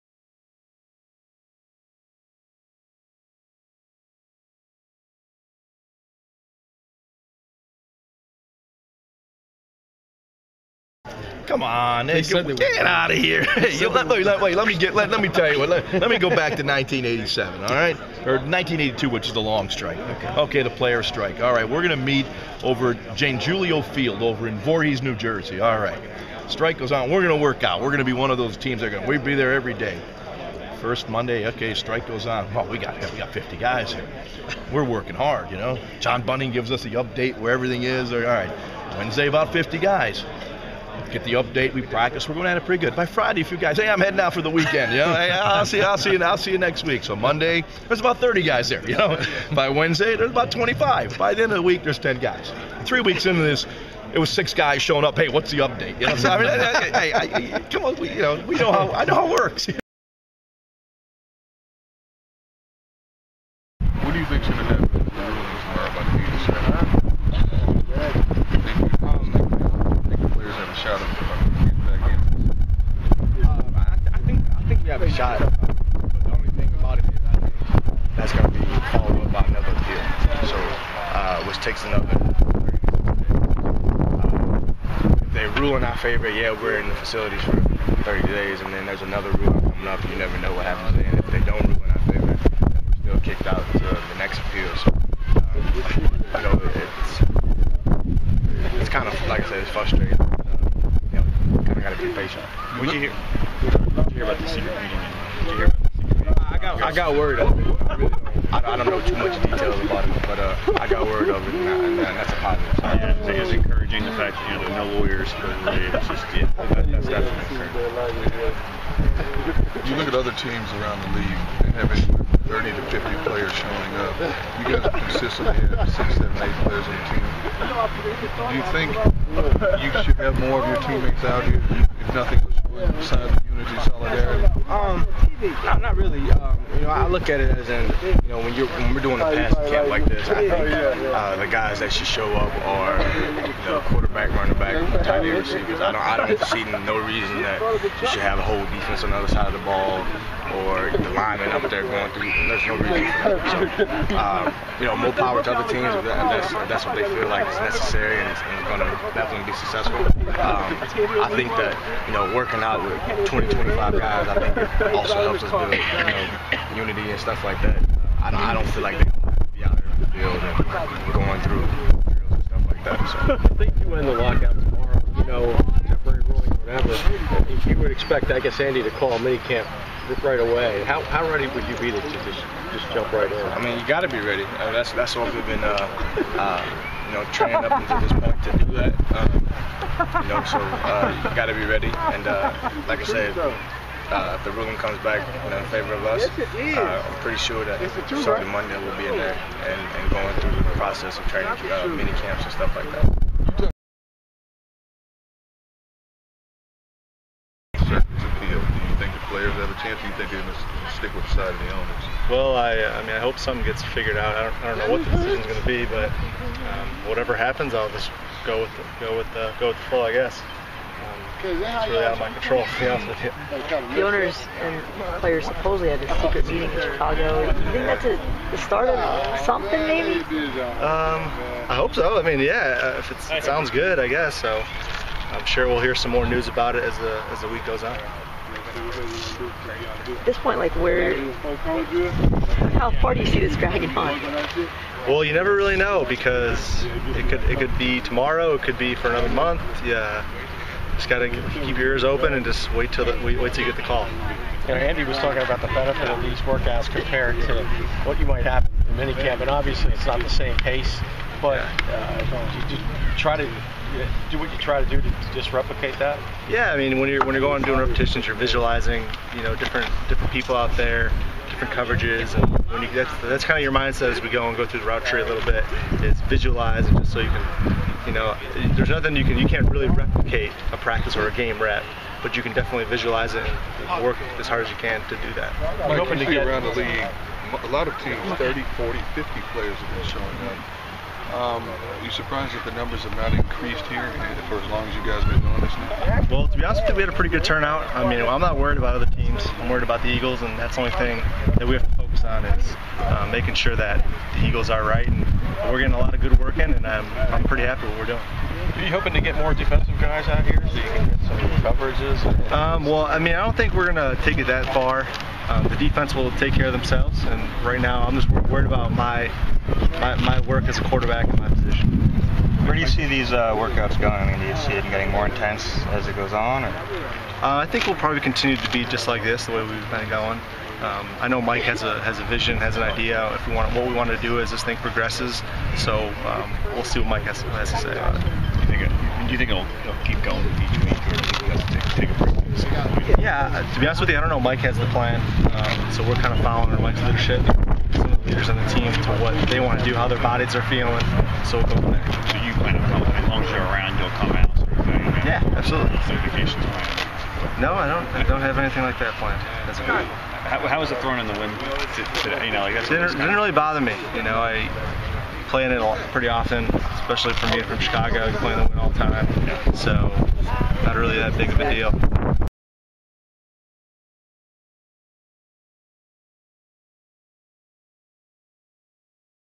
GPM Eagles Video: Workout Turnout
Eagles quarterback Mike Kafka and wide receiver Jeremy Maclin speak after a players workout at Evesham Memorial Sports Complex, plus a bonus comment from former Eagles QB Ron Jaworski from a press conference in early March.